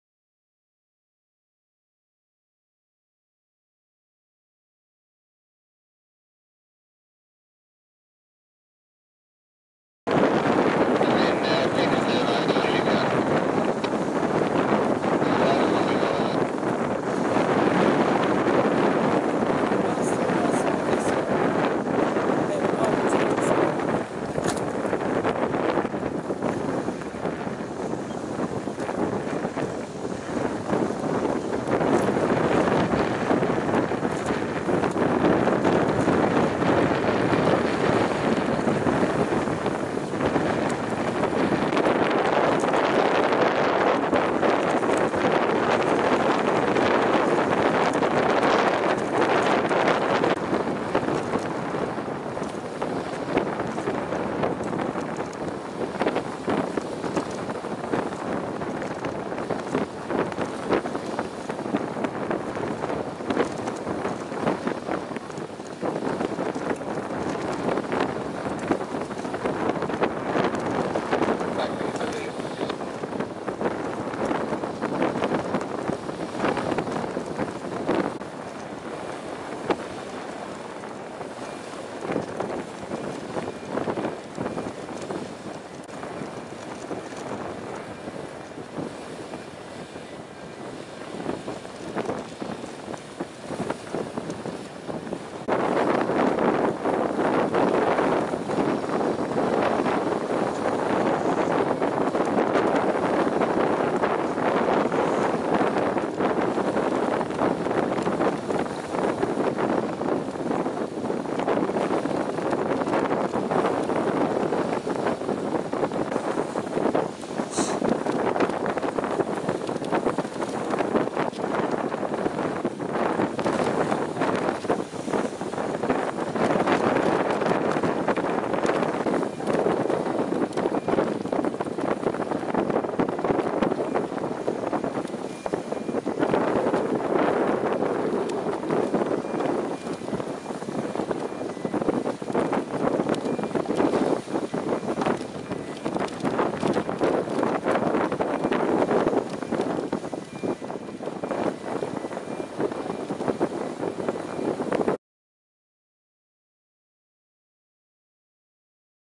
container ship NORTHERN MONUMENT arriving at the port of felixstowe 14/7/16
300m vessel NORTHERN MONUMENT arriving at felixstowe in the morning sunshine.